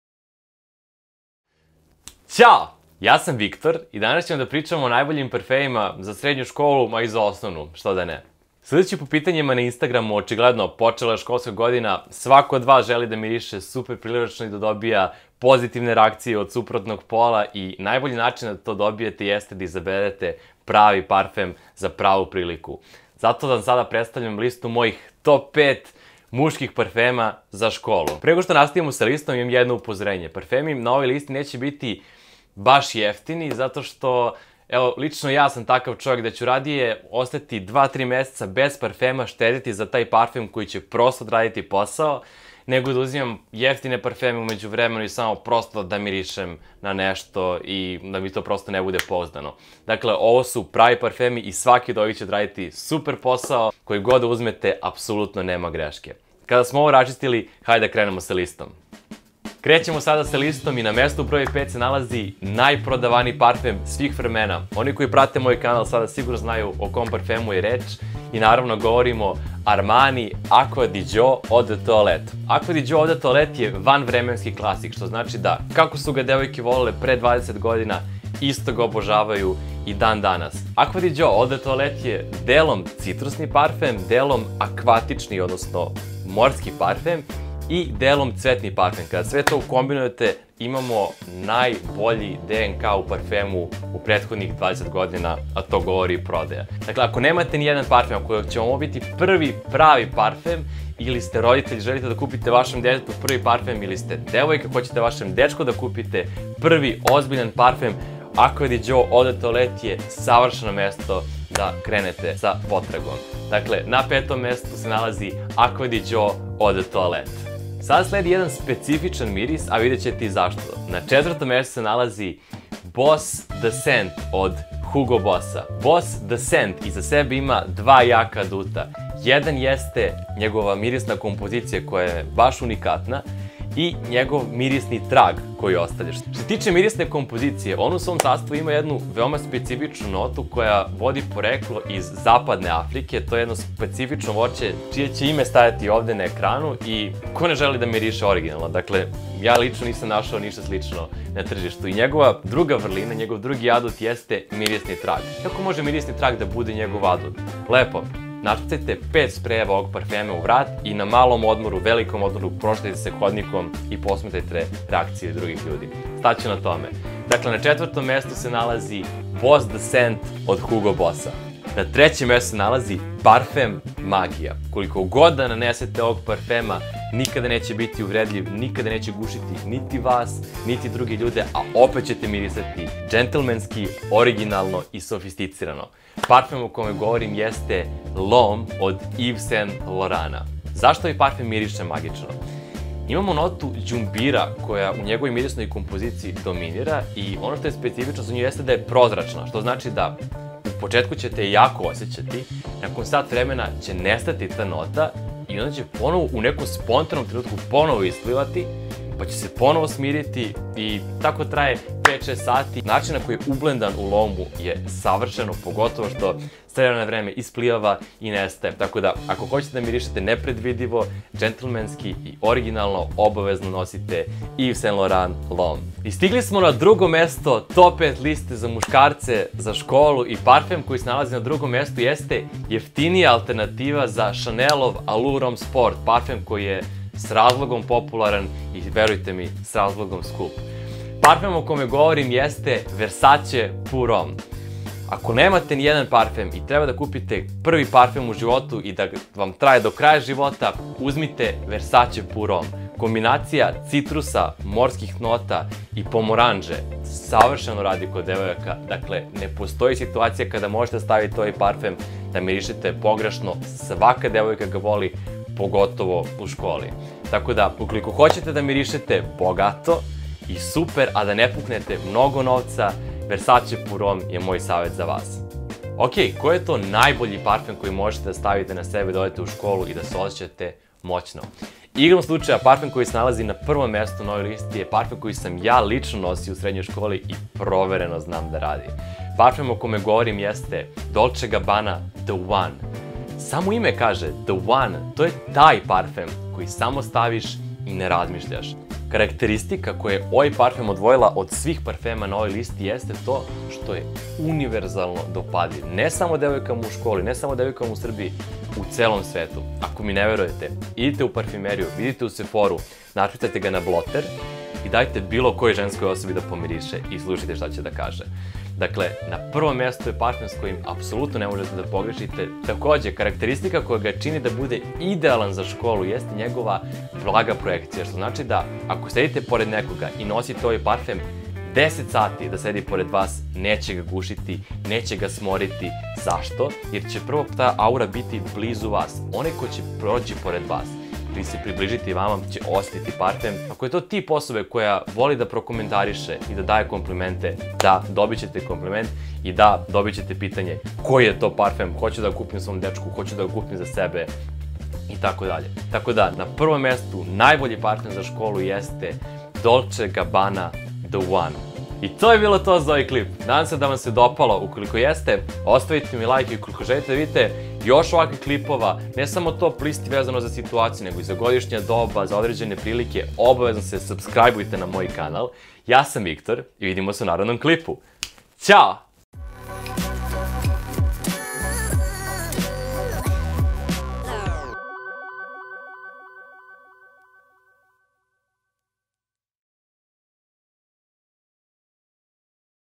Top 5 Parfema za Povratak U Skolu | Back to School najbolji parfemi
Ove nedelje sam vam spremio top 5 listu najboljih parfema za povratak u skolu. Koji god parfem da uzmete od od ovih necete pogresiti, ovi parfemi prosto rade.